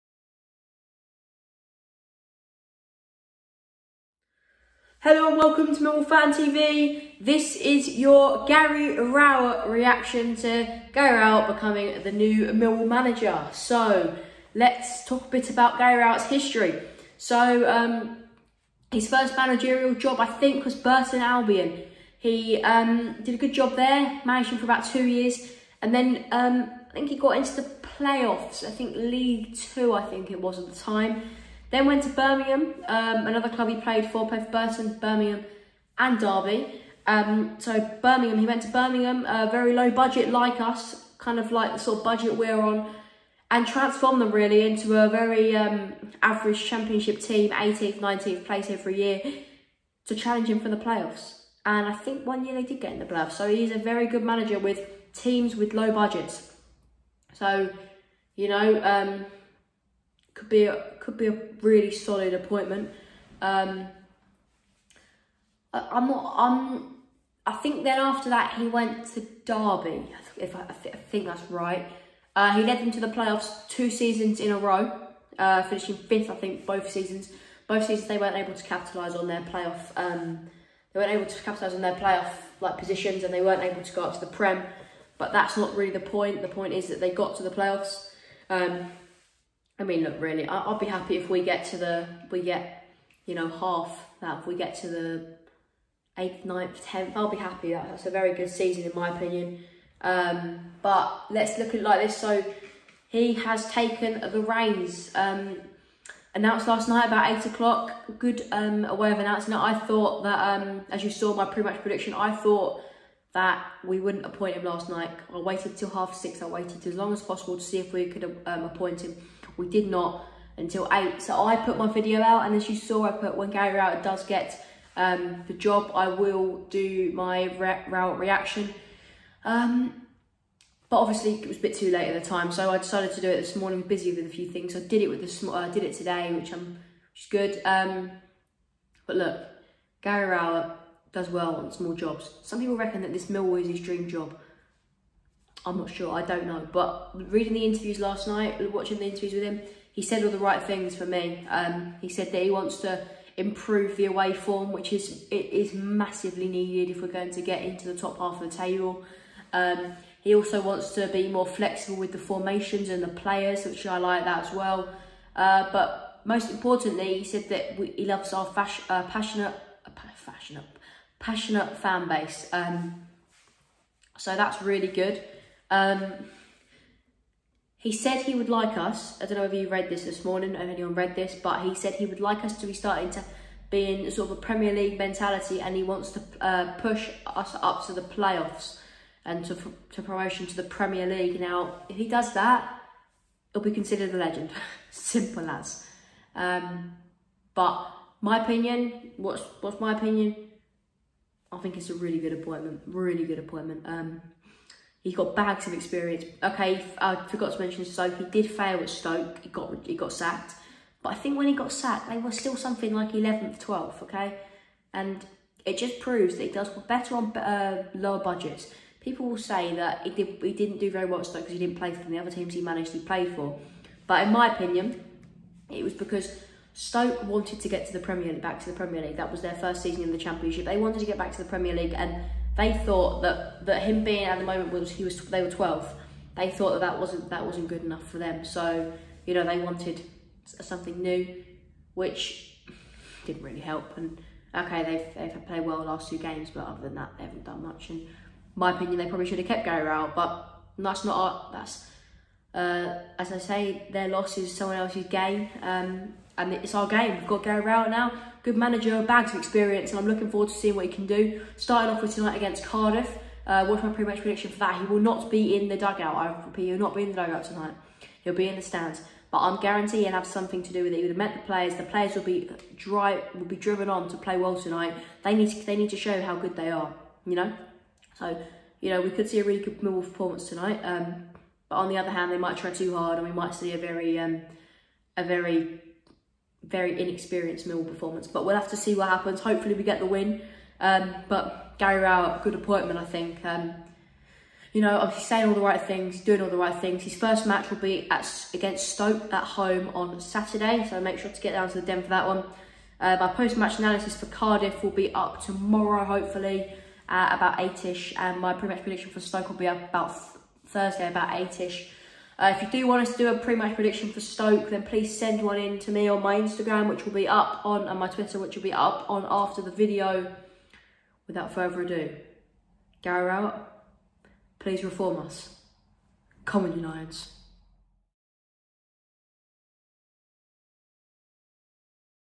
GARY ROWETT REACTION ‘’CAN ROWETT REFORM US’’
Reaction to Gary Rowett being appointed the new Millwall manager! Come on you Lions!